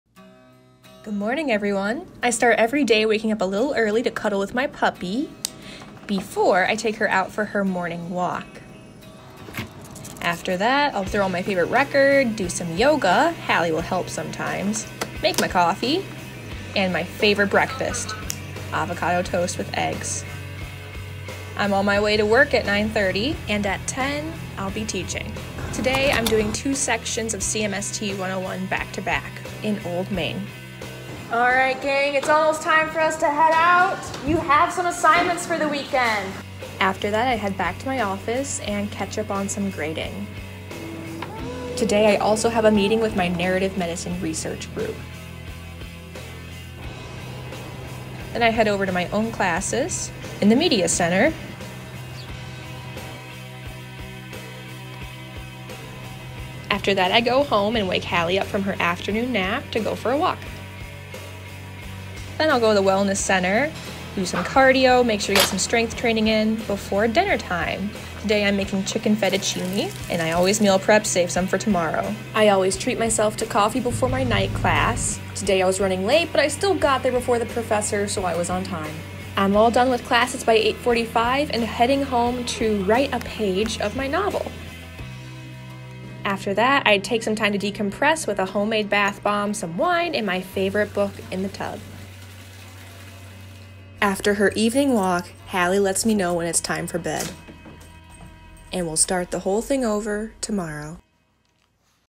A Day in my Life as a USD Grad Student
Follow my day from beginning to end as a second-year communication studies graduate student and graduate teaching assistant at the University of South Dakota. Go Yotes!